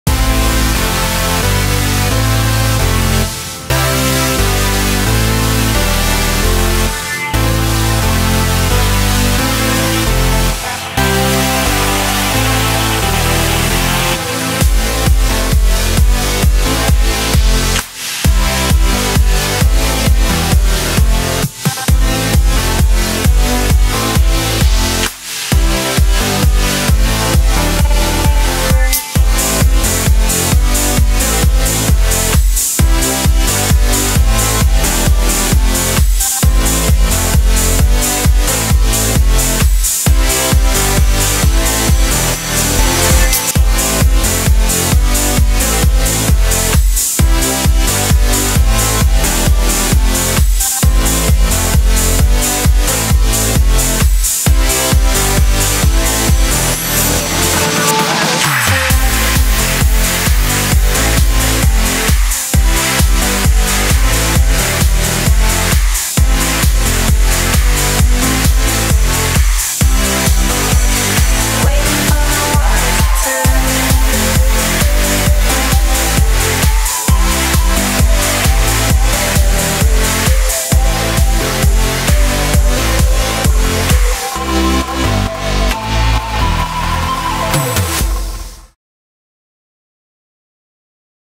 ANDY MOOR @ Open Space by Anna Lee, Kiev FORSAGE DANCE CLUB 23 feb 2013
Anna Lee Presents: OPEN SPACE - British Edition [Special Guest: ANDY MOOR] UK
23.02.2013 Kiev (UA) @ FORSAGE DANCE CLUB